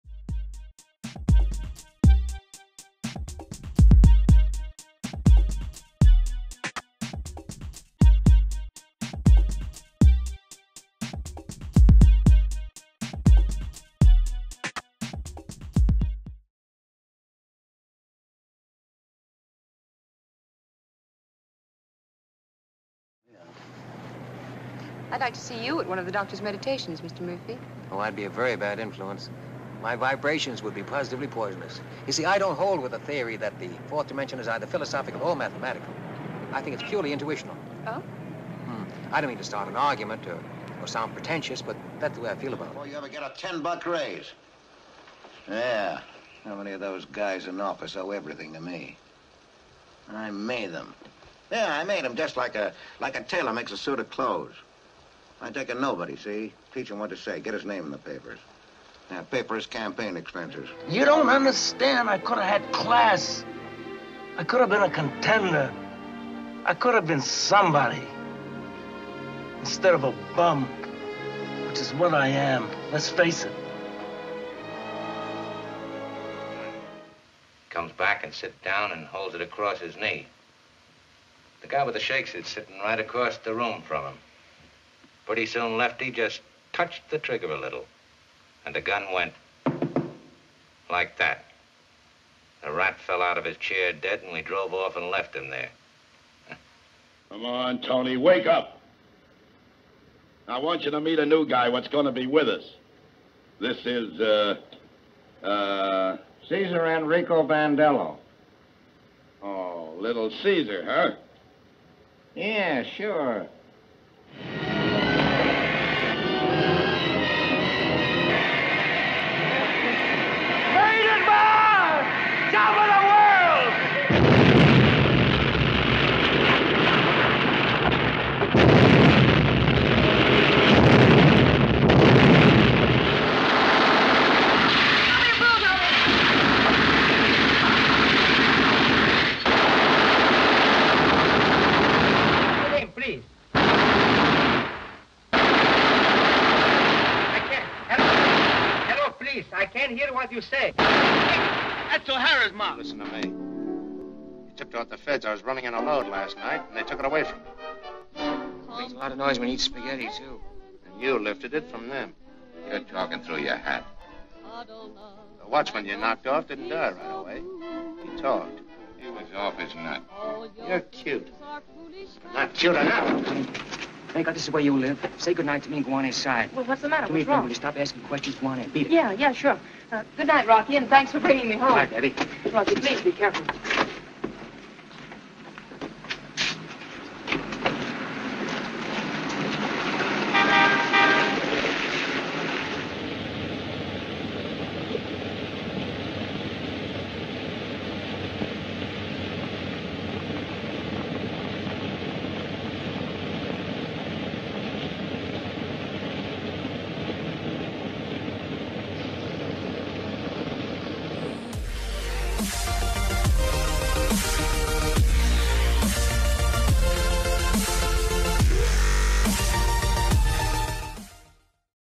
Top 10: Black and White Gangster Movies
We rank the top black and white gangster movies. Most of these contain the man, the myth, the MAVERICK James Cagney, and of course, Humphrey Bogart. Tales that illustrate the rise and fall of the American gangster, as well as the price they have to pay throughout their journey. Enjoy and let us know below if your list would be different.